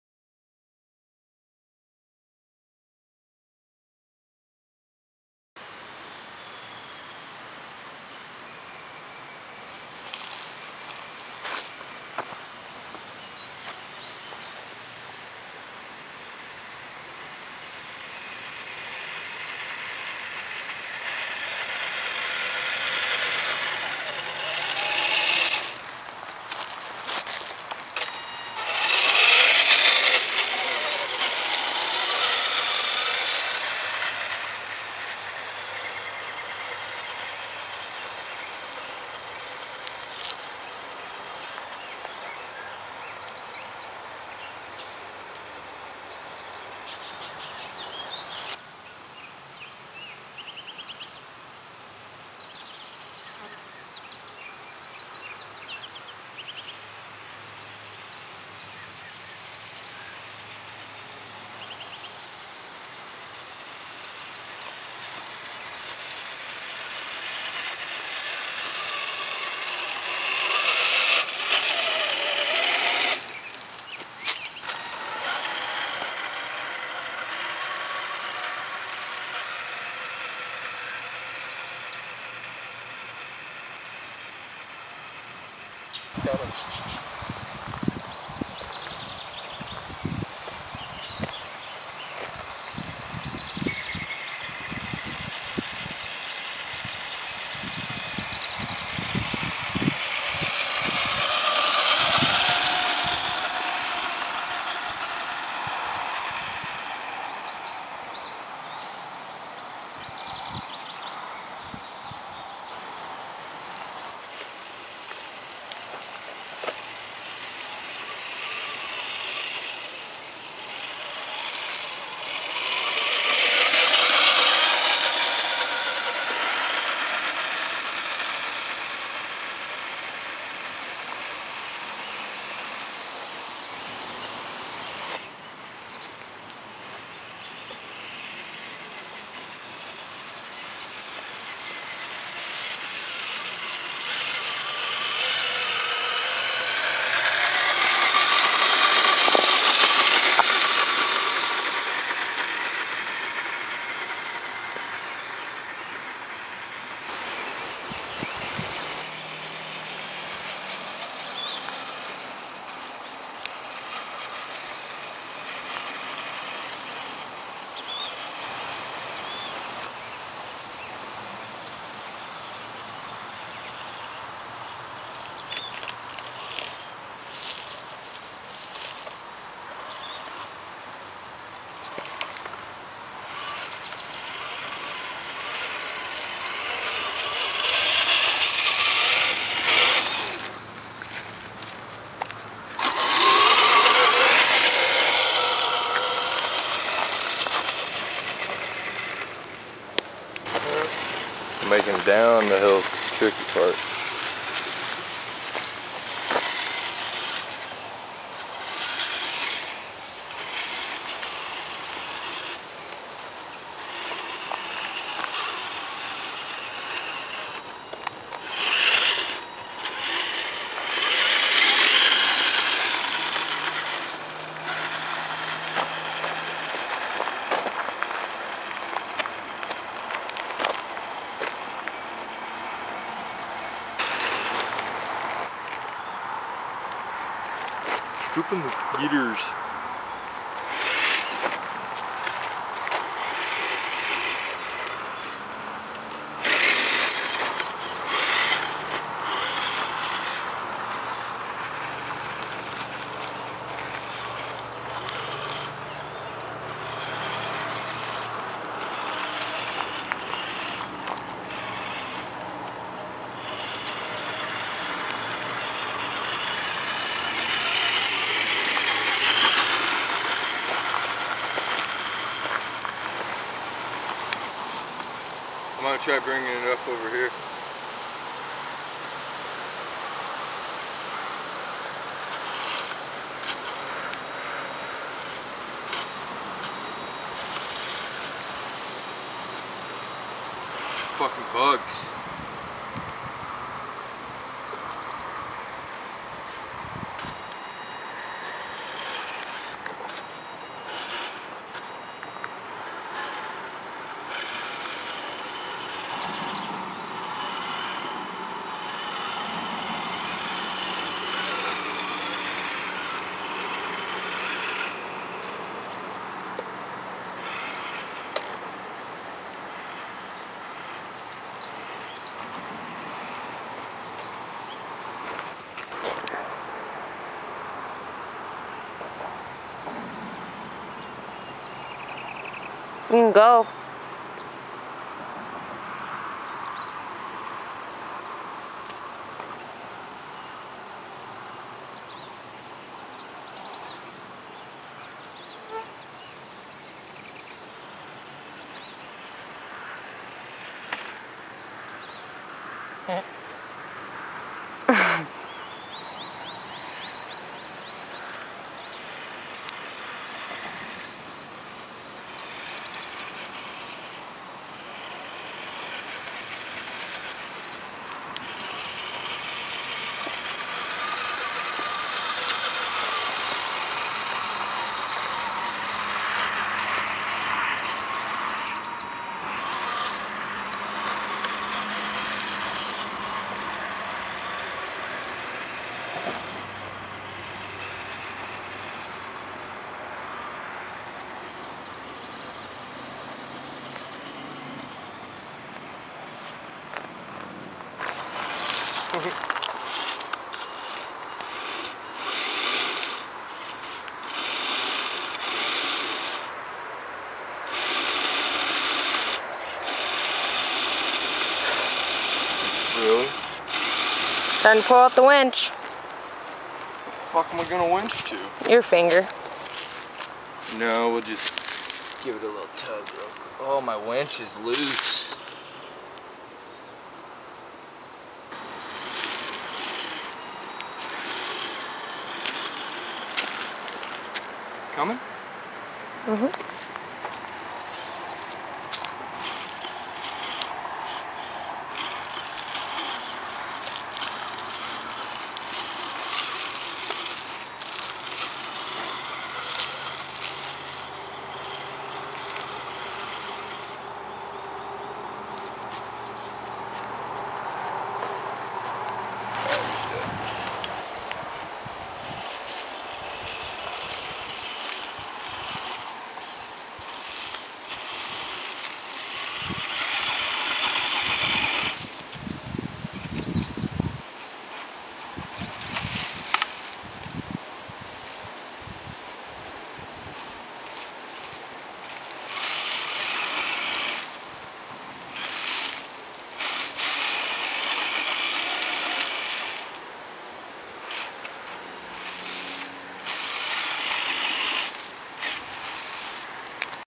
Rc tamiya tundra trail run
my second trail run with the new tundra had a few probs with the winch and bugs lol